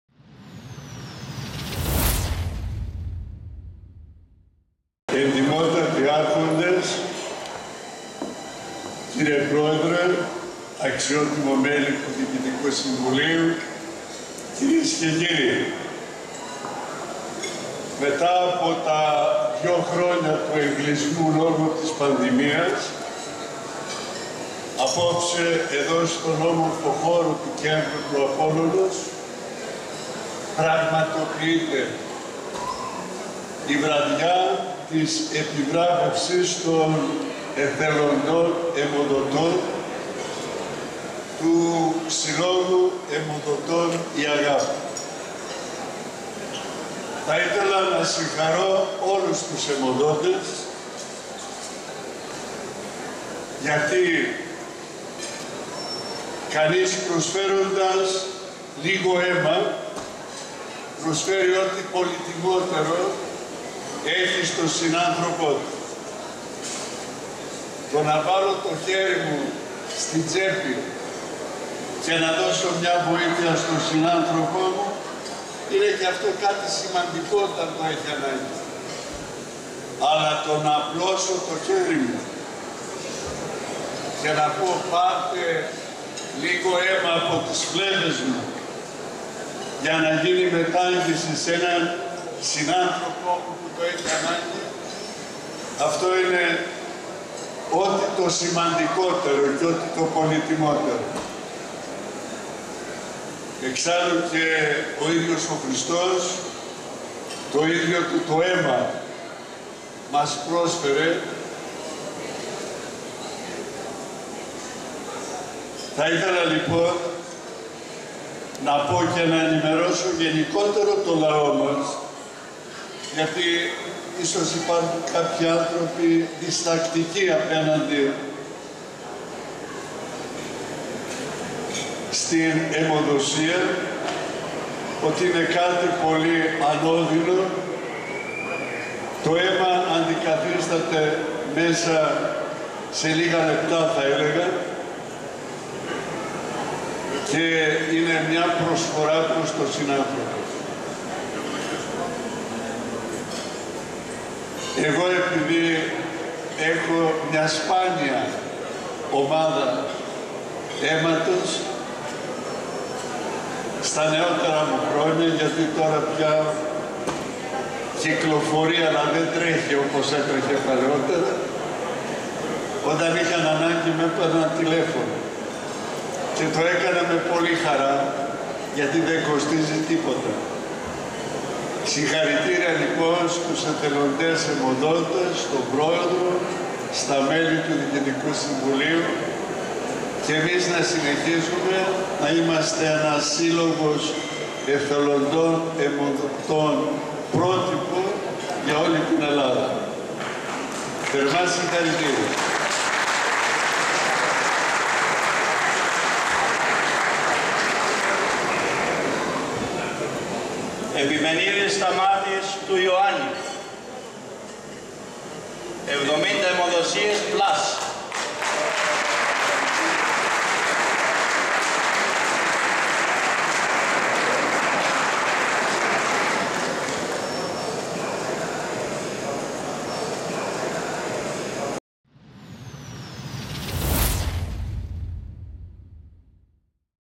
ΜΗΤΡΟΠΟΛΙΤΗΣ ΞΑΝΘΗΣ: ΑΚΟΛΟΥΘΩΝΤΑΣ ΤΟ ΠΑΡΑΔΕΙΓΜΑ ΤΟΥ ΧΡΙΣΤΟΥ ΠΟΥ ΜΑΣ ΠΡΟΣΦΕΡΕ ΤΟ ΙΔΙΟ ΤΟΥ ΤΟ ΑΙΜΑ
ΜΗΤΡΟΠΟΛΙΤΗΣ ΞΑΝΘΗΣ : ΑΚΟΛΟΥΘΩΝΤΑΣ ΤΟ ΦΩΤΕΙΝΟ ΠΑΡΑΔΕΙΓΜΑ ΤΟΥ  ΧΡΙΣΤΟΥ ΠΟΥ ΜΑΣ ΠΡΟΣΦΕΡΕ ΤΟ ΙΔΙΟ ΤΟΥ ΤΟ ΑΙΜΑ , ΑΣ ΠΡΟΣΦΕΡΟΥΜΕ ΤΗΝ ΑΓΑΠΗ ΜΑΣ ΠΡΟΣ ΤΟΝ ΣΥΝΑΝΘΡΩΠΟ